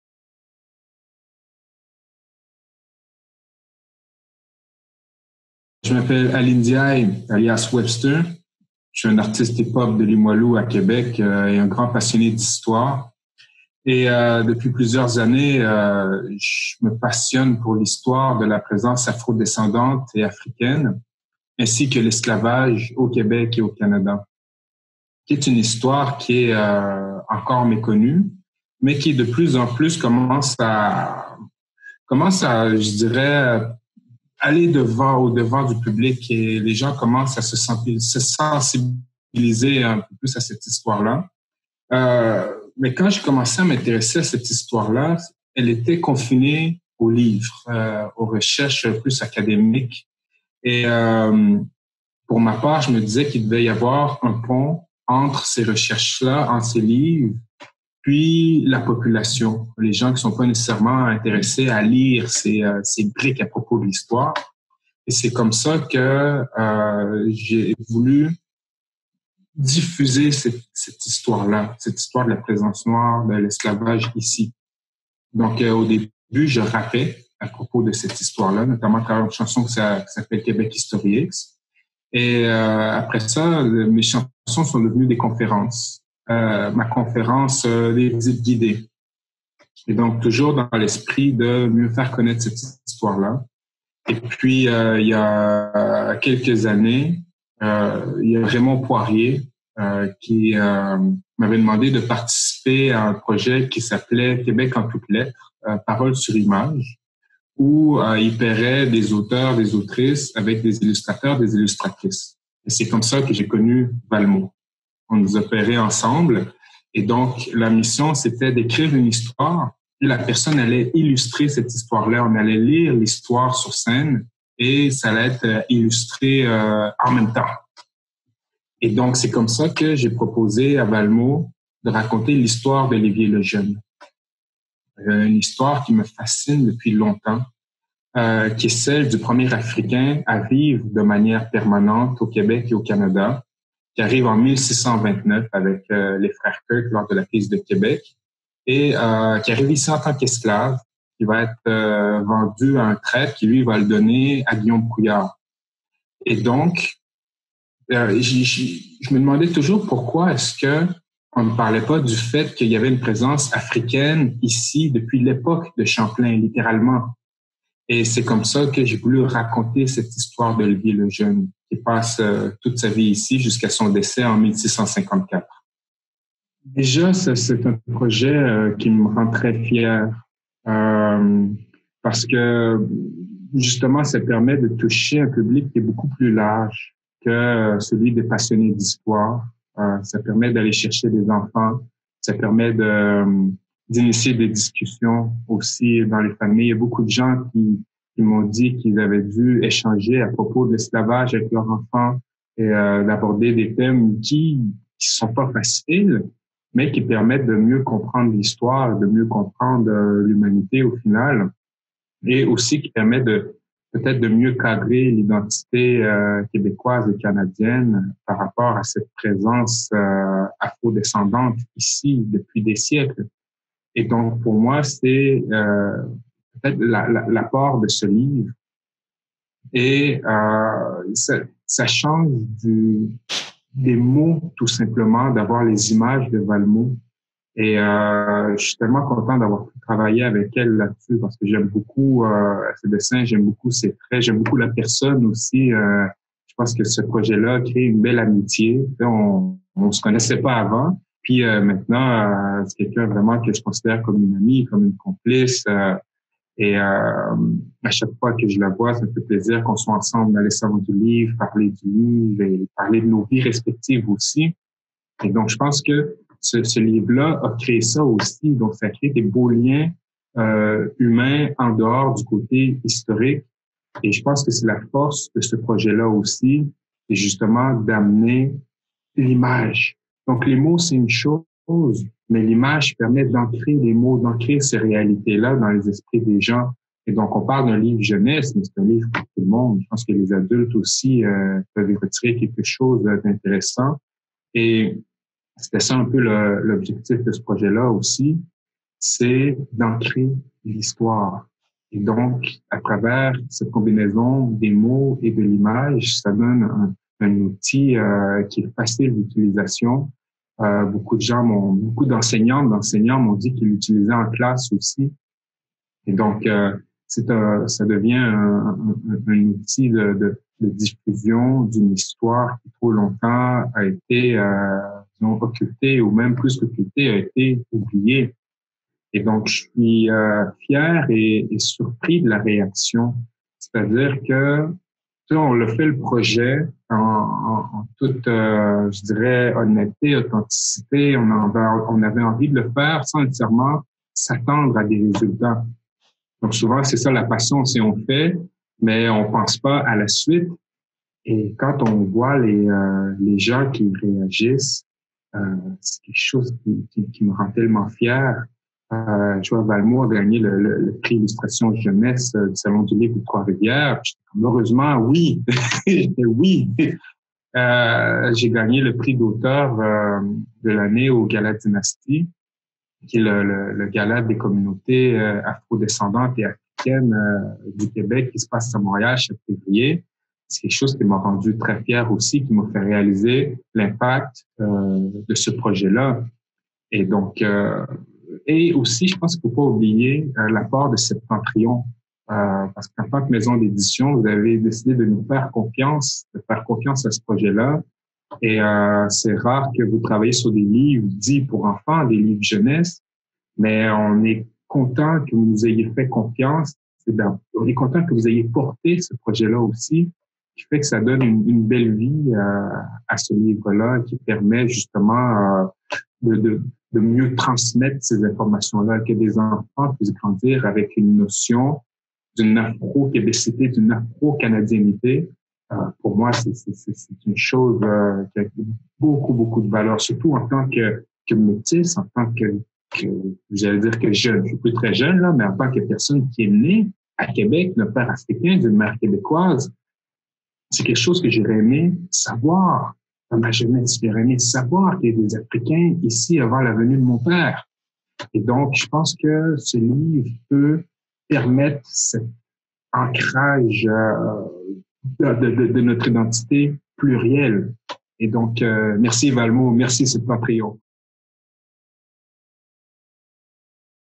Webster (auteur) présente «Le Grain de sable»
Webster présente son plus récent livre: «Le Grain de sable. Olivier Le Jeune, premier esclave au Canada»

Originaire de l'île de Madagascar, Olivier Le Jeune arrive dans la ville de Québec en 1629 en tant qu'esclave. Il avait 10 ans.

Au-delà de son statut d'esclave, il est la première personne d'origine africaine à habiter de manière permanente au Canada. Ce livre, inspiré de ce qu'il a réellement vécu, suit le parcours d'Olivier Le Jeune, de sa capture à Madagascar jusqu'à son arrivée dans la ville de Québec. On apprend comment il a pu s'adapter à sa nouvelle réalité dans les débuts de la Nouvelle-France.